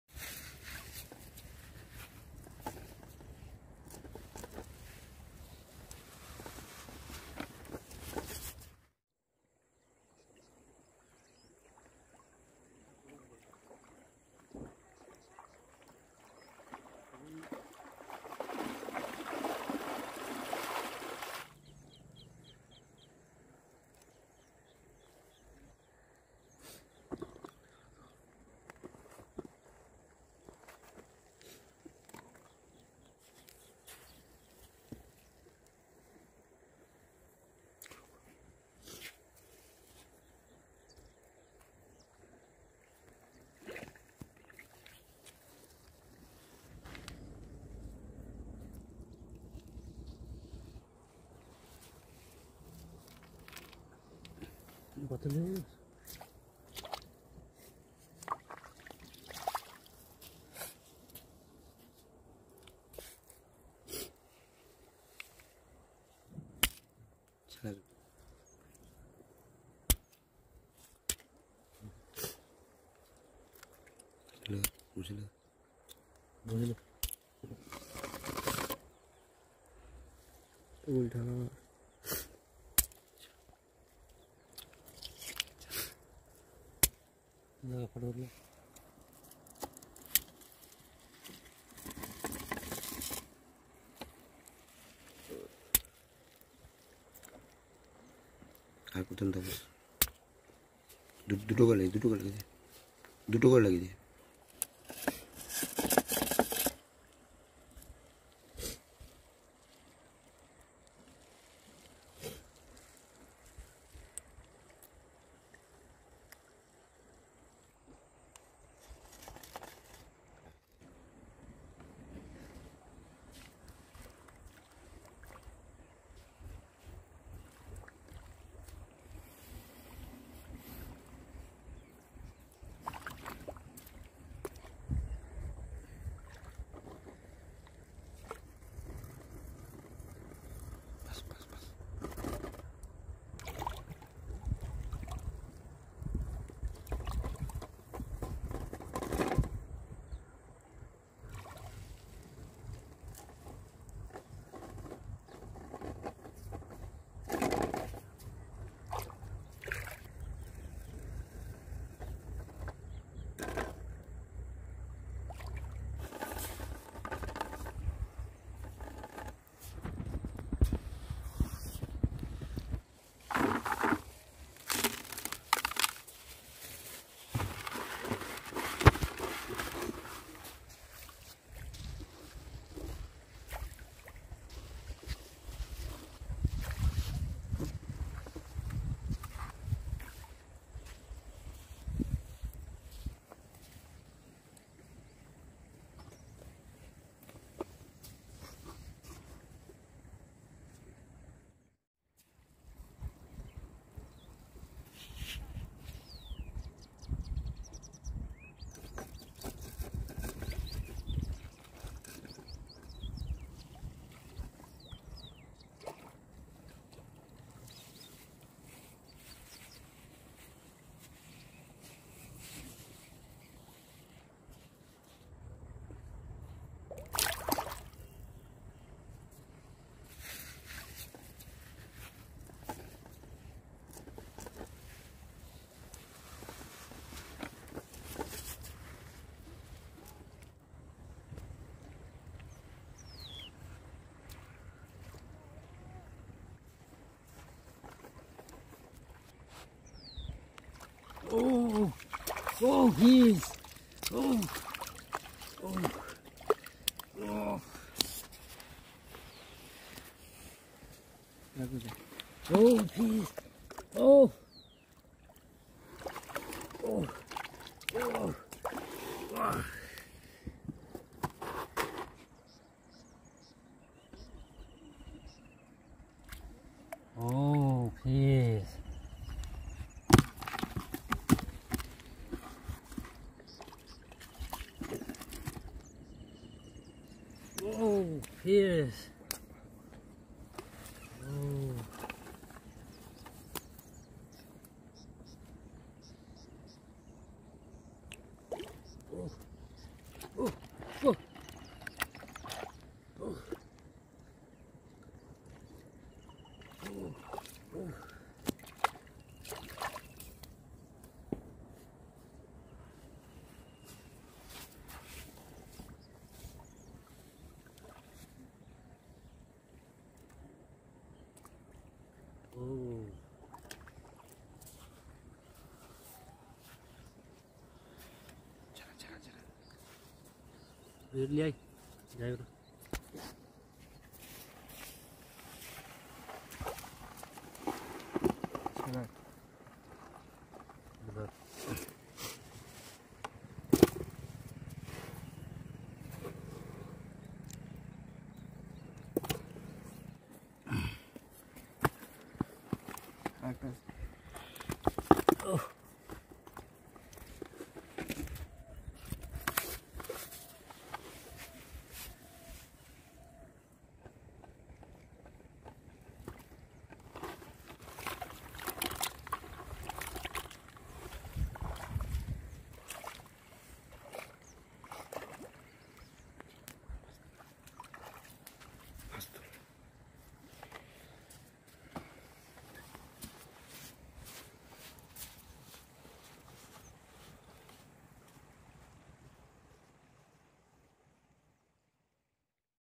Unique hook fishing trap. Hook fishing trap with bamboo. Bamboo trap for riverside fishing .
Unique hook fishing trap. Hook fishing trap with bamboo.  Bamboo trap riverside fishing. An amazing fishing video presented from Fishing and Trapping channel.
In this video we use riverside  small oyster  as fish baits.  Then spread rice and mustard cake to attract fish. We wait for one hour and watch fish hooked  one by one.
We collect those fish . Hope you have enjoyed our Natura fishing video.
That's all.